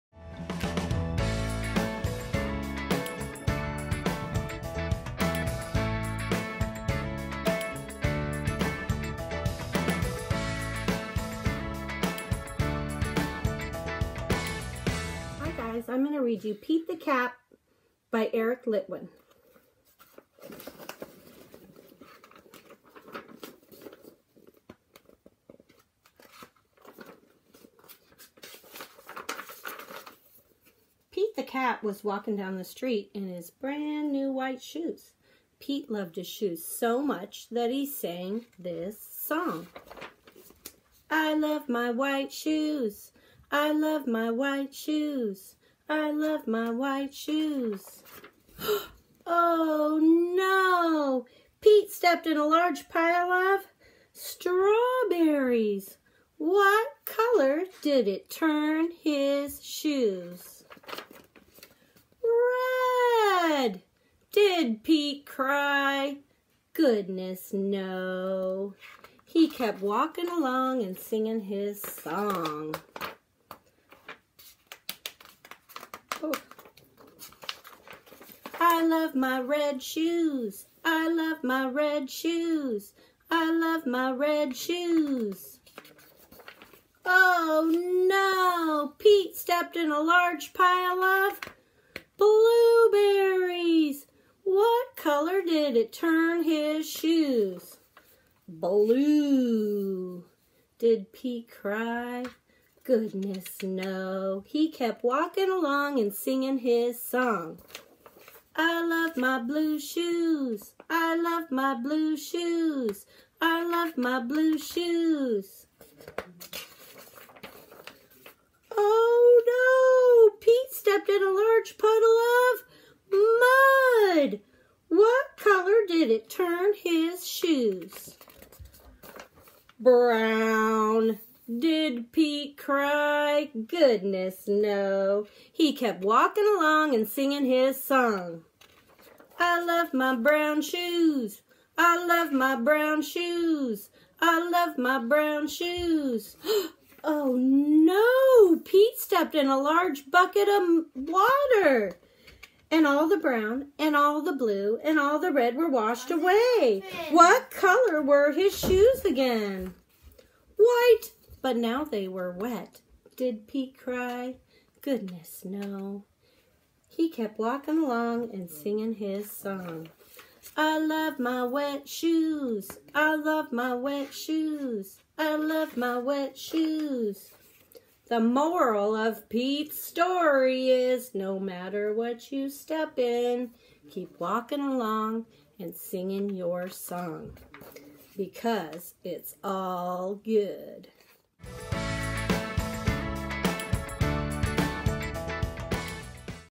Miss Nina Story Time! - Pete the Cat
It's Pete the Cat! Shared with us by Miss Nina!